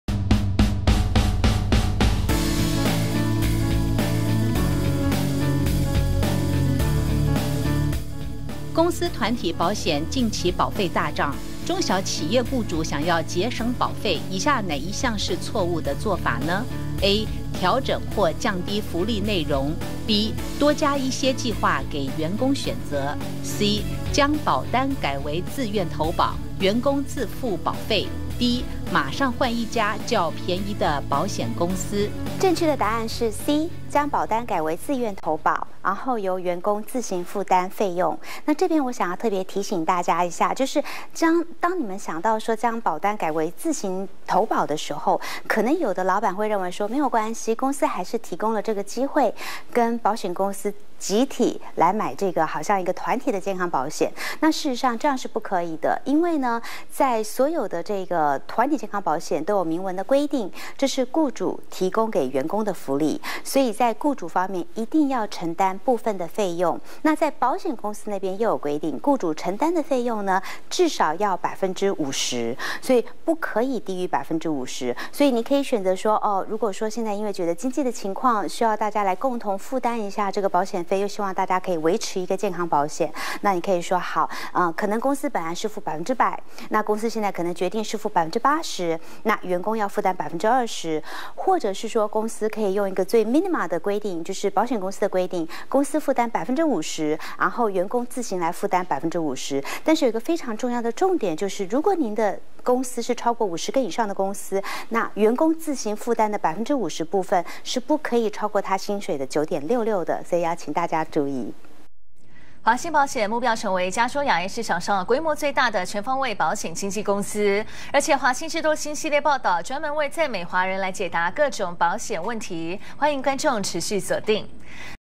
雇主想節省保費+保障員工福利？華興保險教你怎麼做！
「新冠病毒」疫情衝擊全美經濟，華人企業主 如何兼顧 保費 與 員工福利？東森新聞 特別聯手「華興保險」，由「團體健康保險、規劃師」、深入說明。